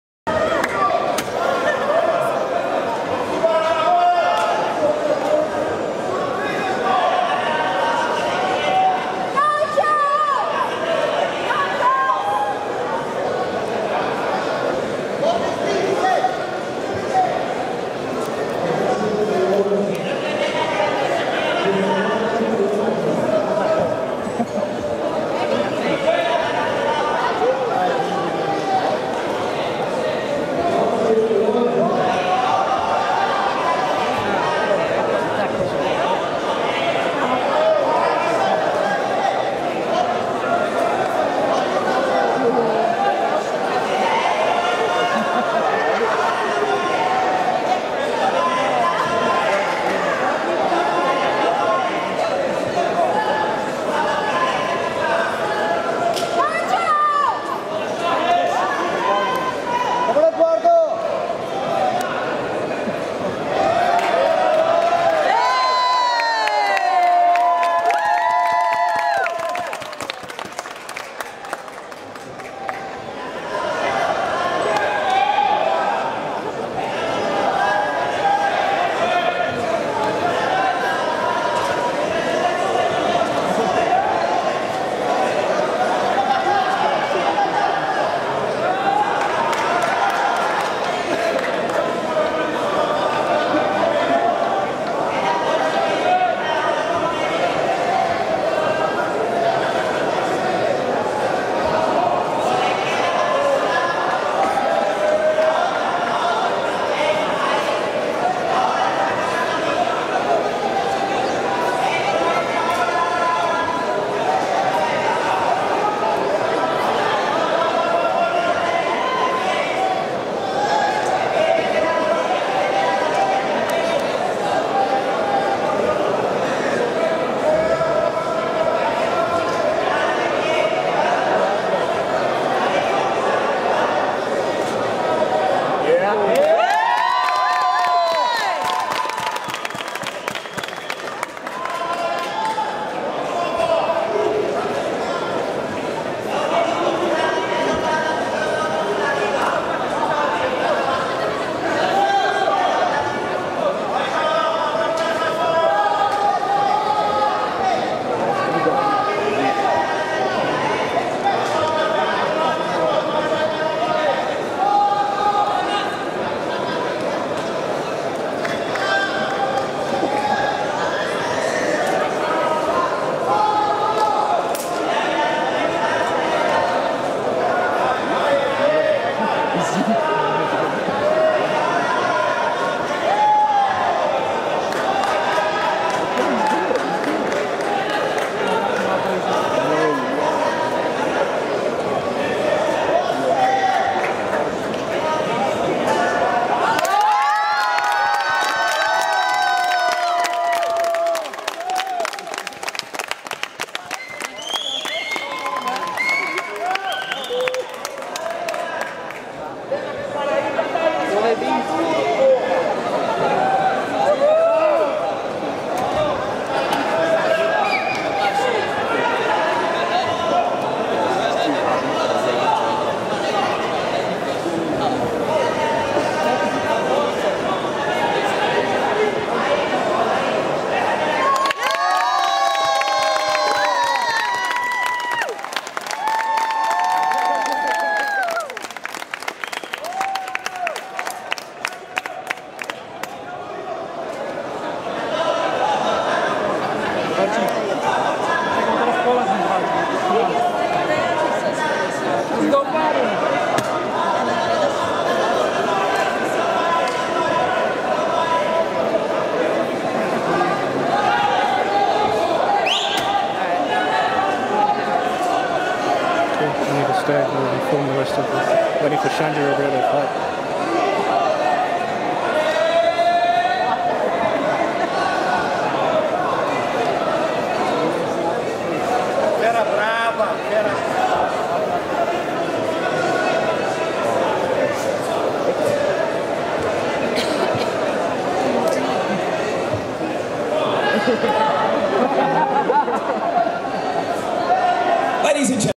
Charles Negromonte vs Santeri Lilius - FINAL - Abu Dhabi Grand Slam London 2017 - Black Adult
85kg:
Charles Negromonte (Roger Gracie Academy) vs Santeri Luis (Gracie Barra)